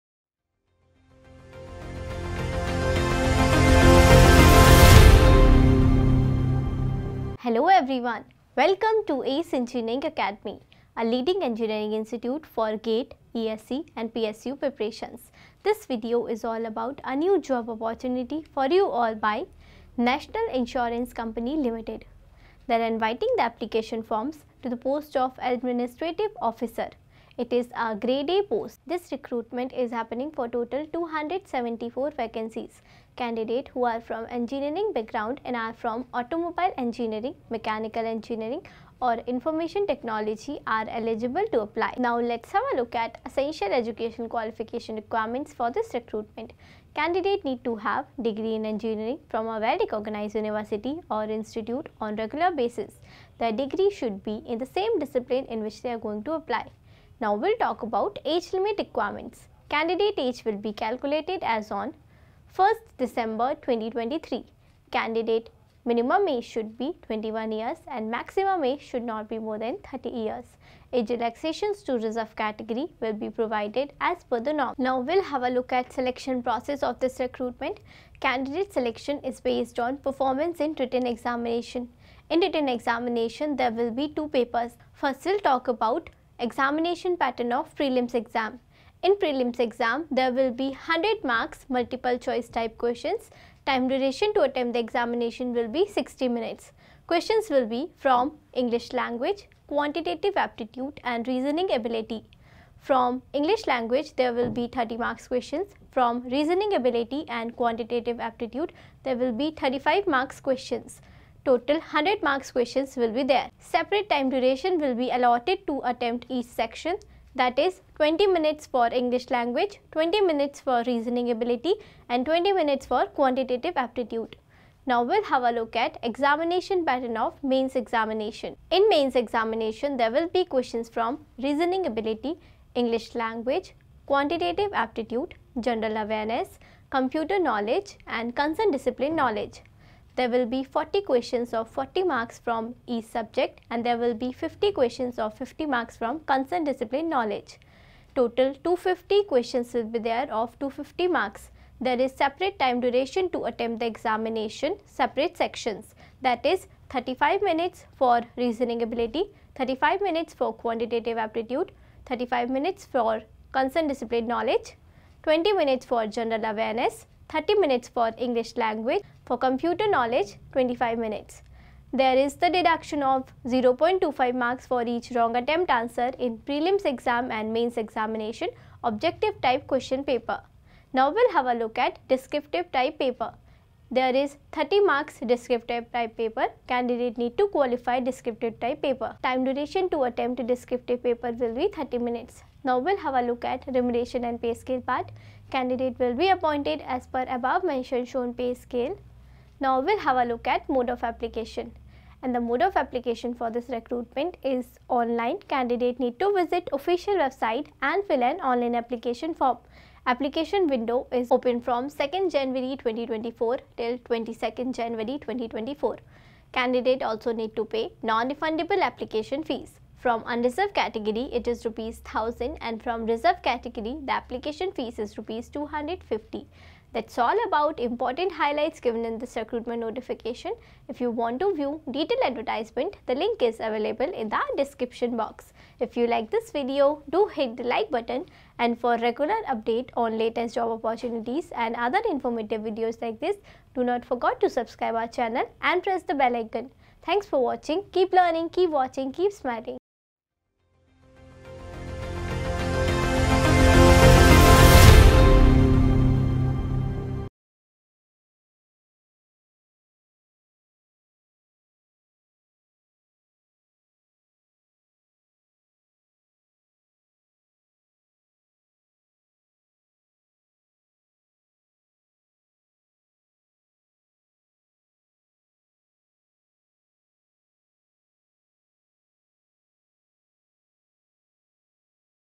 NICL invites applications to the post of Admin. Officers To Grade- A cadre | Last date: 22/01/2024
National Insurance Company Limited invites the application form to the post of Administrative Officers To Grade- A cadre

POST NAME: Administrative Officers

DISCPLINE
 Automobile Engineering
 Mechanical
 Information technology

EDUCATIONAL QUALIFICATION
 Full Time Degree in Engineering from a recognized university/Institute

AGE LIMIT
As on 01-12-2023
21-30 Years
Age Relaxations as per Norms

SELECTION PROCESS
Written Exam

EXAM PATTERN
Preliminary Exam: (English: 30 marks questions in 20 minutes, Quantitative Aptitude: 35 marks questions in 20 minutes,
Reasoning: 35 marks questions in 20 minutes)
 Time Duration: 60 minutes
 Total marks: 100
 1/4th marks negative marking for wrong answer

Mains Exam:
 Reasoning: 40 Questions/40 Marks in 35 Minutes
 English: 40 Questions/40 Marks in 30 Minutes
 General Awareness: 40 Questions/40 Marks in 20 Minutes
 Computer Knowledge: 40 Questions/40 Marks in 25 Minutes
 Quantitative Aptitude: 40 Questions/40 Marks in 35 Minutes
 Subject Knowledge: 50 Questions/50 Marks in 35 Minutes
Descriptive Test: 30 Marks question in 30 Minute

PAY SCALE
Basic pay would be Rs. 50,925 /-

Mode: Online
Start date : 02/01/2024
Last date: 22/01/ 2024

Application fees:
General: 1000.- Rs
SC/ST/PwD: 250 Rs